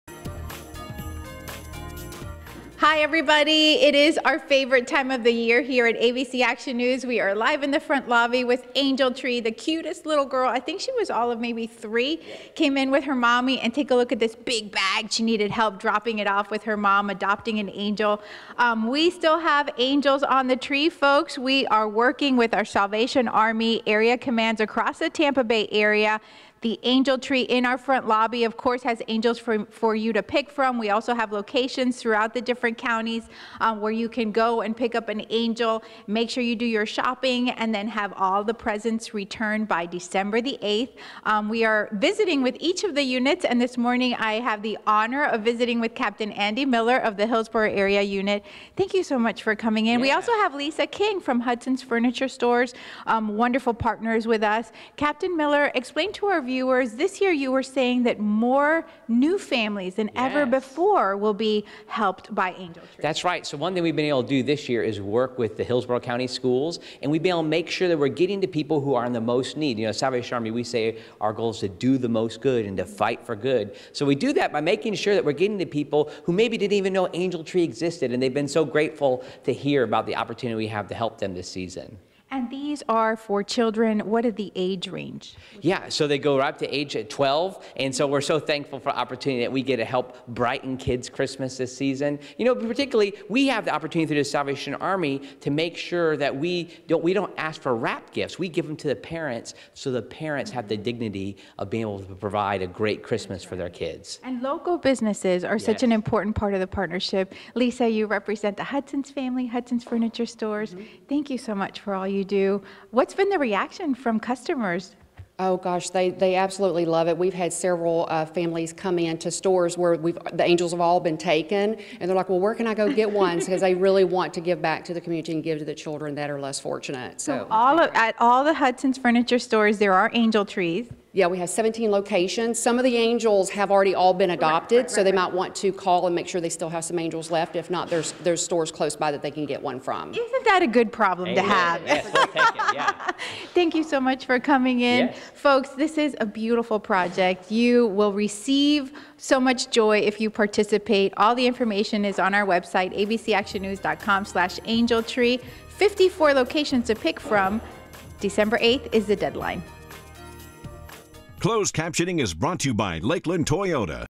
Positively Tampa Bay: Angel Tree
Hillsborough county's Salvation Army unit says 90% of its Angel Tree families this year are receiving help for he first time! The need is great and Hudson's Furniture Showrooms across FL are hosting angel trees to help!